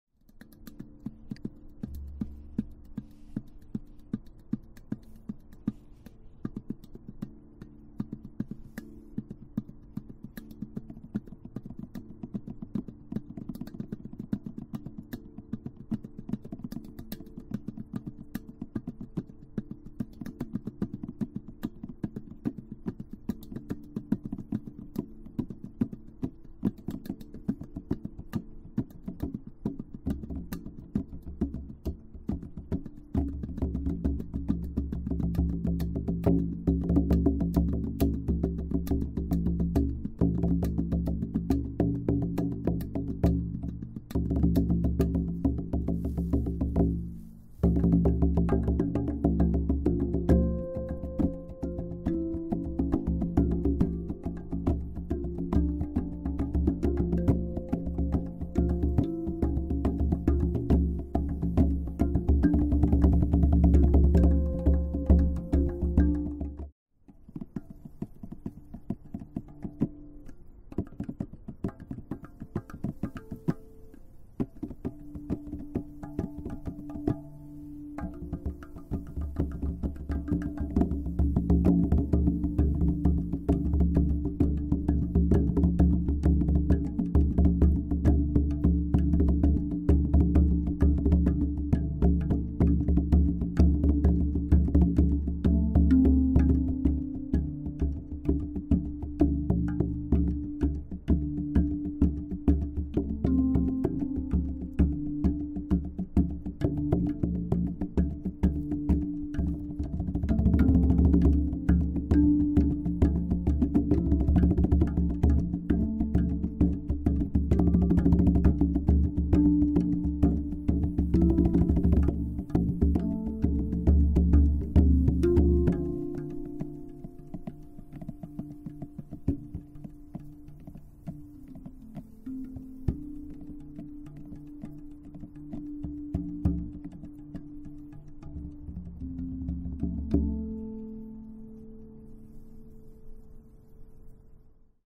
Buffalo Drum & Nova drum Improvisation
Myself and my brother @adammykemusic  improvise at the river! This time I am also playing on his D Celtic minor nova drum. Adam also throws in some throat singing Enjoy!